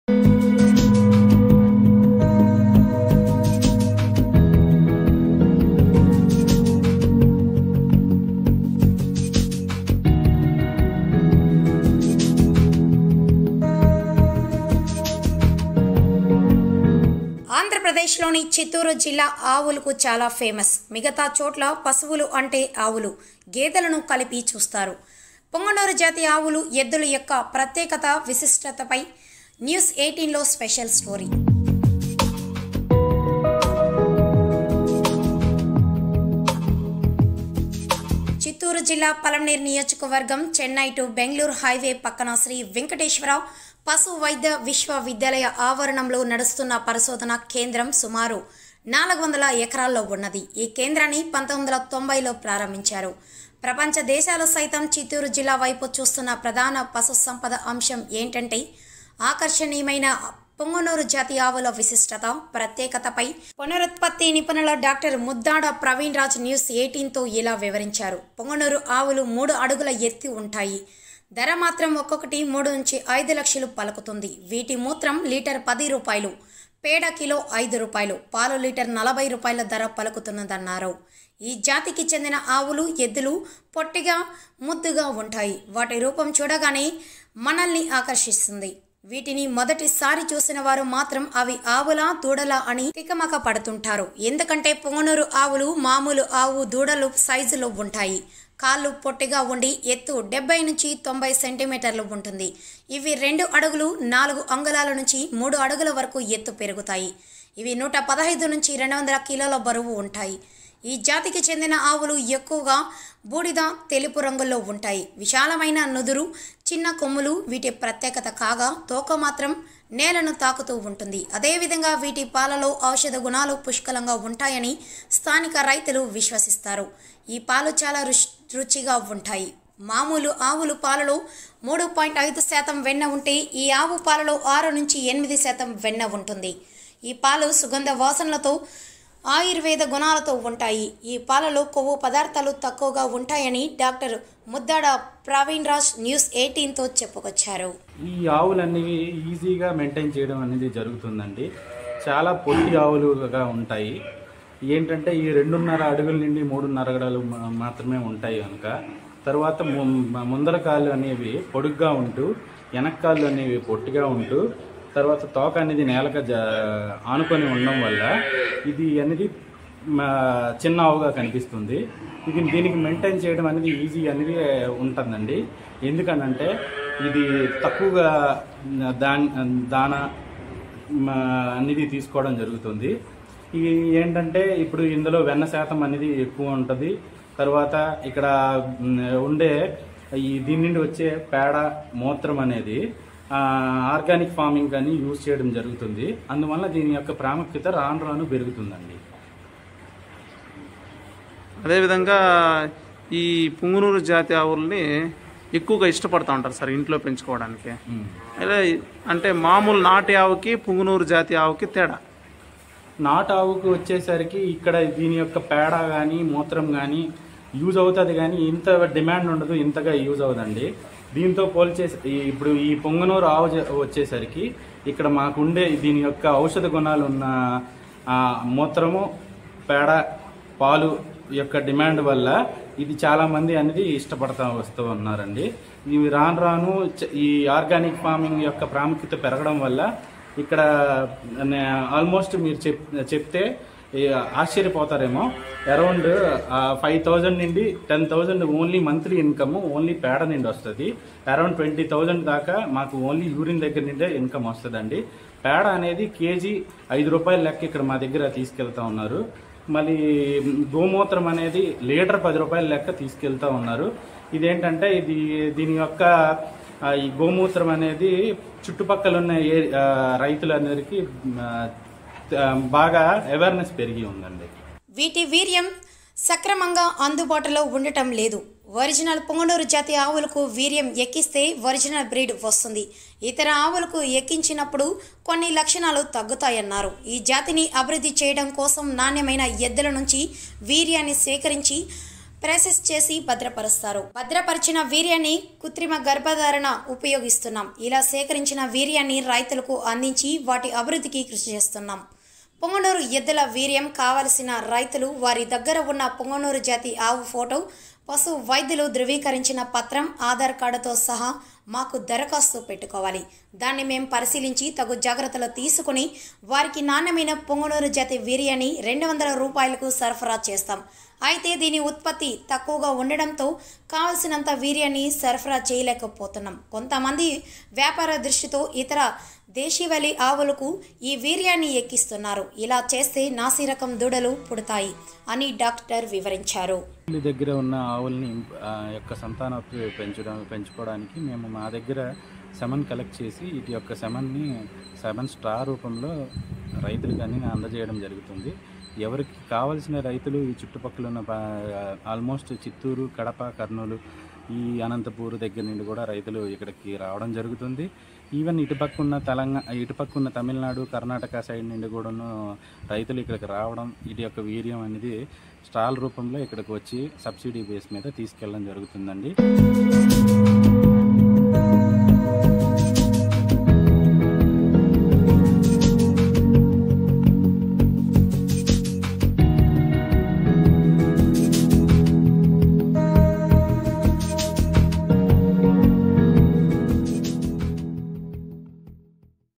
Chittoor : పుంగనూరు జాతి ఆవుల పెంపకంతో లాభాలే లాభాలు...! | Punganur Cow
ఆంధ్రప్రదేశ్‌లోని చిత్తూరు జిల్లా ఆవులకు చాలా ఫేమస్. మిగతా చోట్ల పశువులు అంటే ఆవులు, గేదెలను కలిపి చూస్తారు పుంగనూరు జాతి ఆవులు,ఎద్దులు యొక్క ప్రత్యేకత,విశిష్ఠతపై న్యూస్ 18 లో స్పెషల్ స్టోరీ.